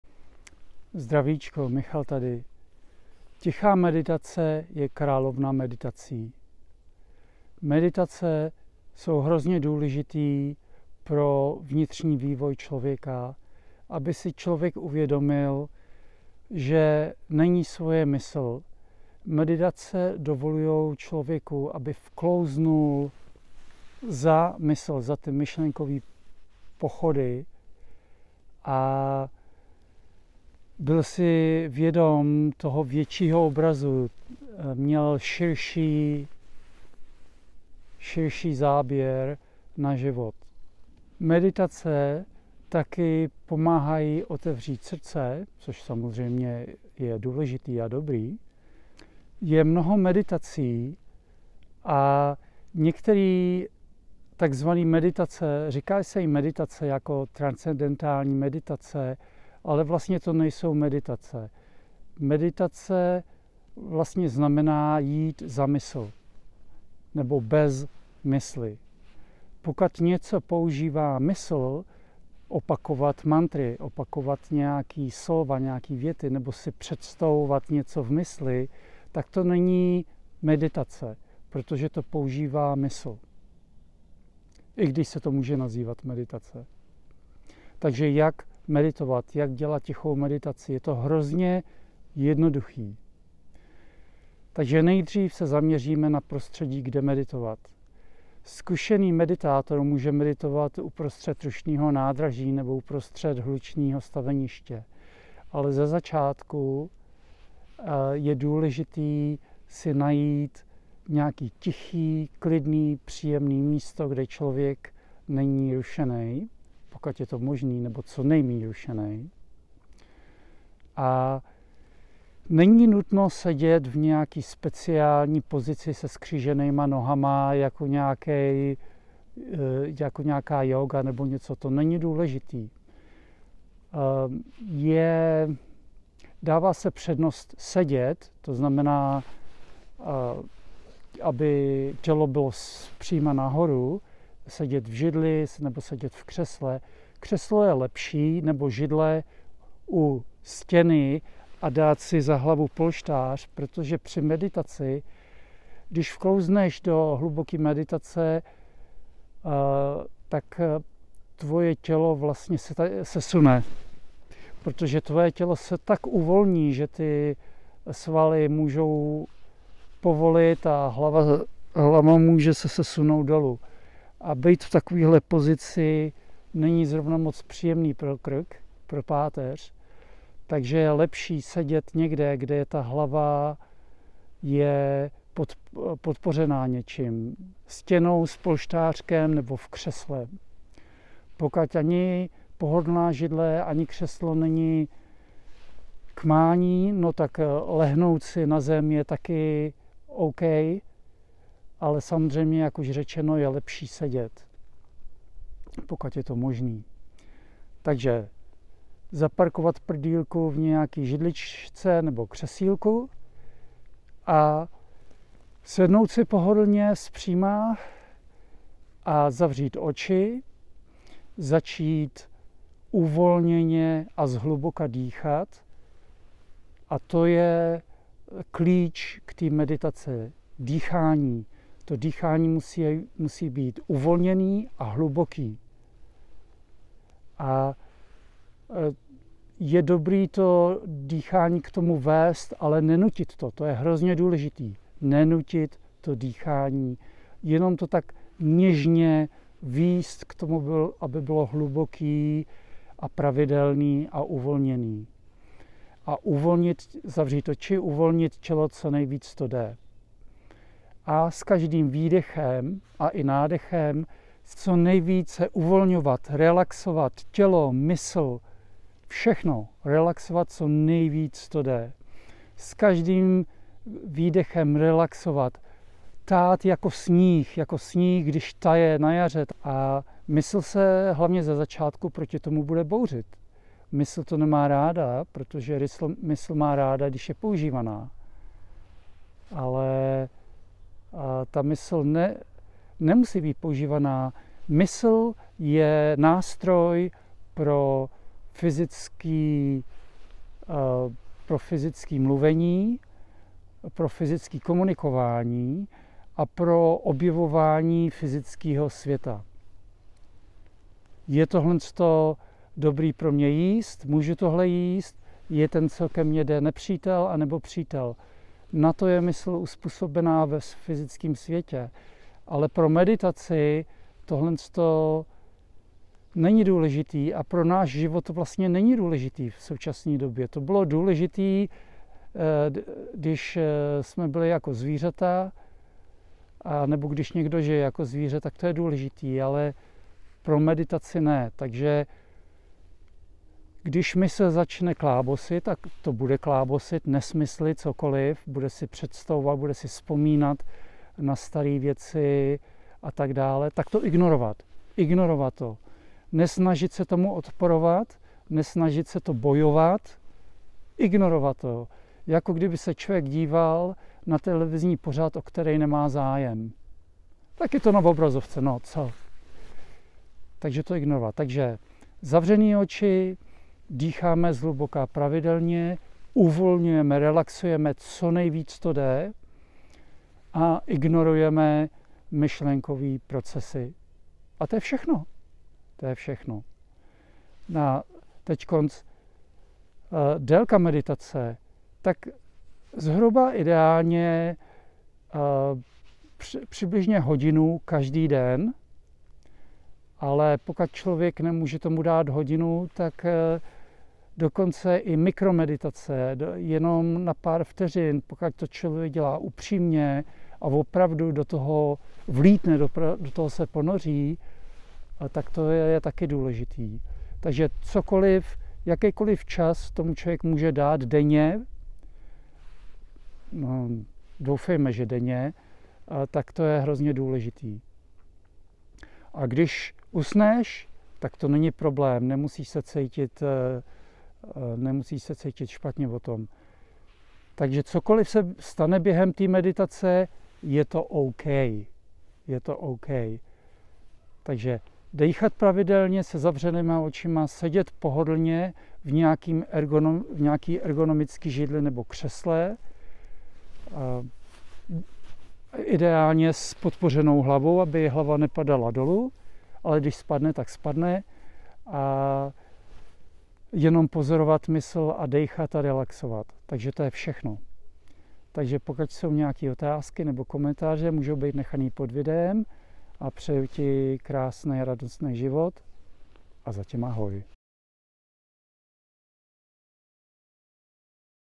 Jak meditovat - tichá meditace, královna meditací..
Návod a porozumění jak meditovat tichou meditaci.  Je to jednoduché a důležité pro zdraví vývoj člobrdy.